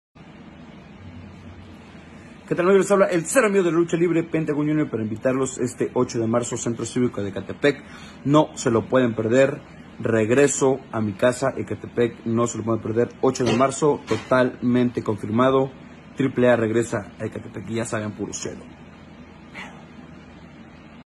Pentagón Jr regresa a Ecatepec para enfrentar a Rush y L.A. Park en compañia de Pagano.
Pentagón Jr regresa al Centro Civico de Ecatepec para enfrentar a Rush y L.A. Park en compañia de Pagano con Promociones González este próximo 8 de marzo.